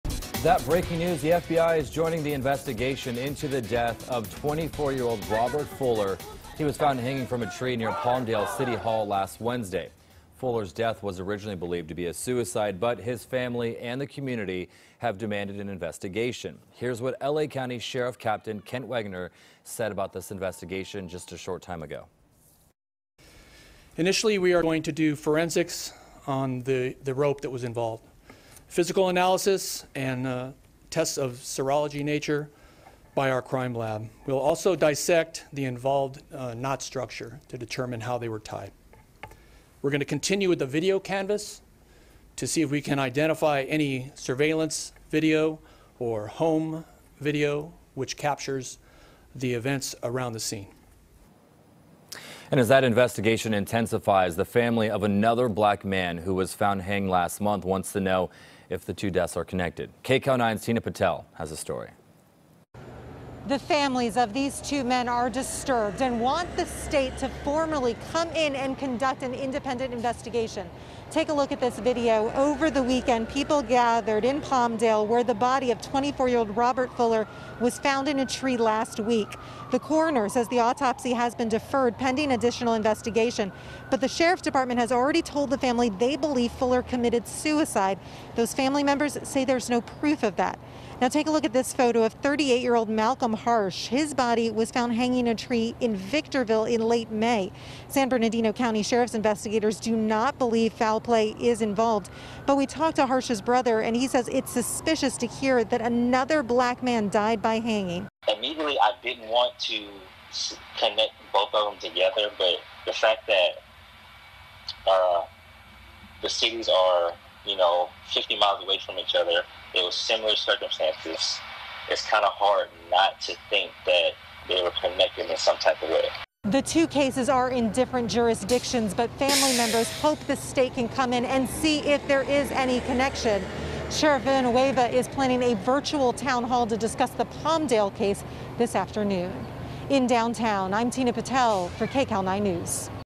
FBI, California AG To 'Monitor' Robert Fuller Death Investigation
Both the FBI Civil Rights Division and the California Attorney General's Office will review the Los Angeles County Sheriff's Department's investigation into the death of Robert Fuller, a 24-year-old black man who was found hanging from a tree in Palmdale last week.